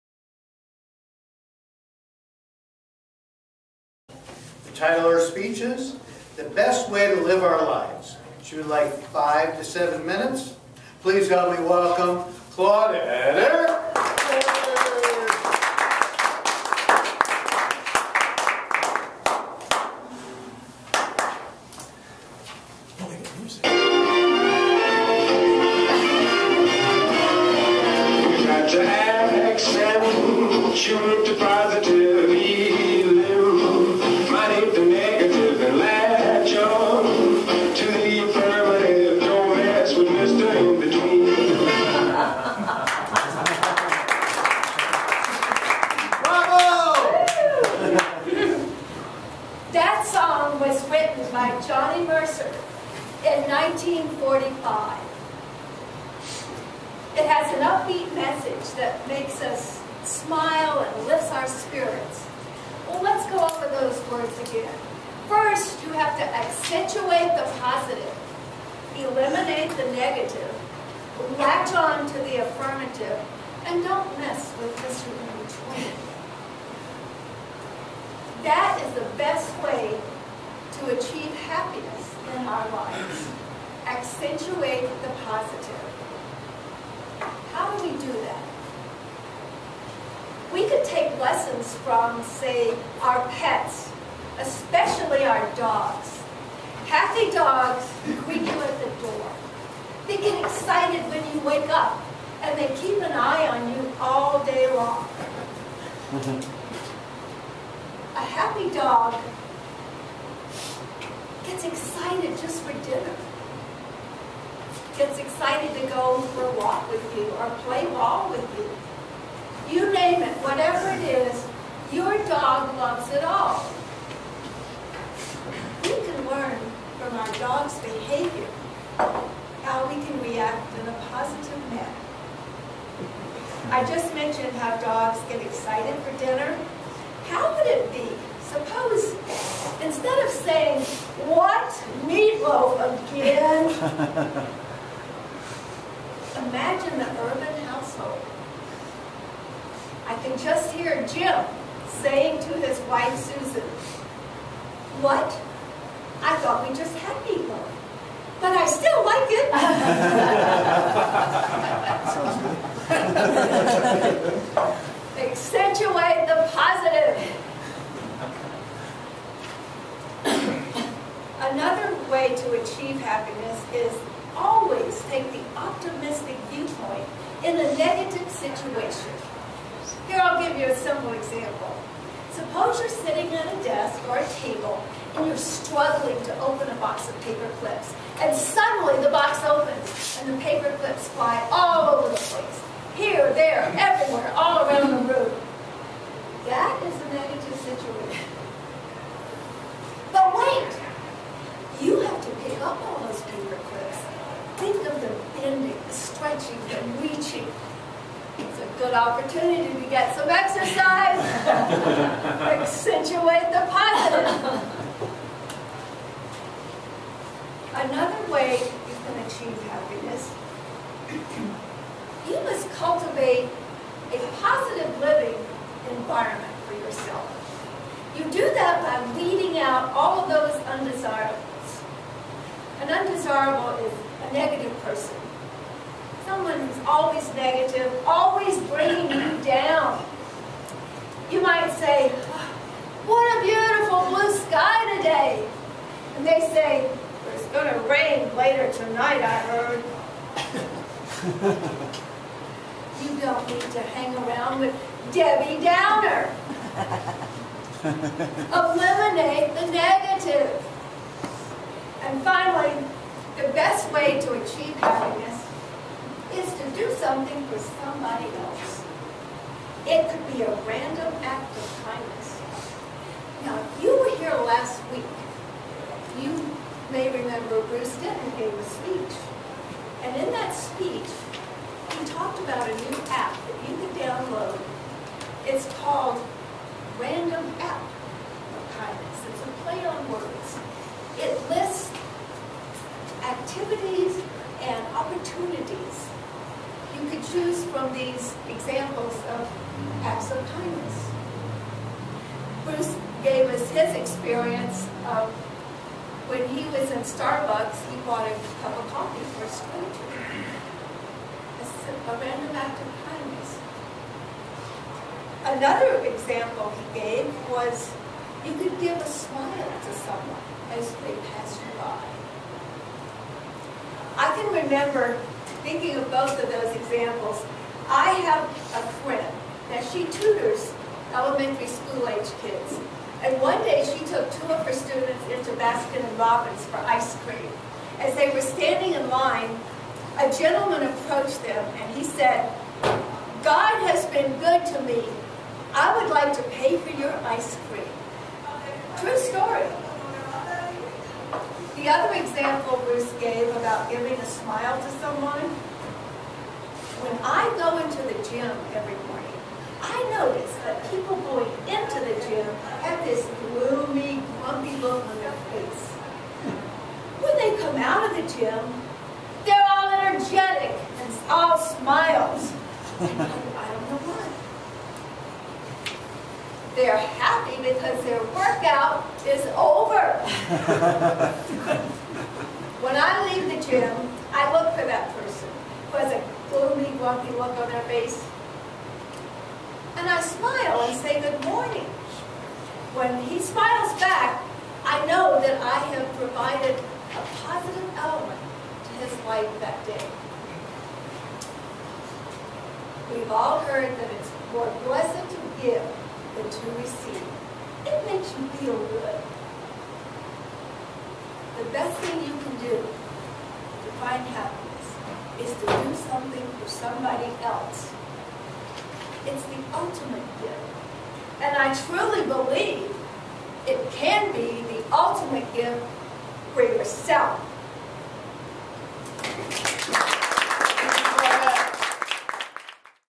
Accentuate The Positive
This video is about public speaking and staying positive and to encourage one to Accentuate The Positive. This is a toastmasters pathways speech.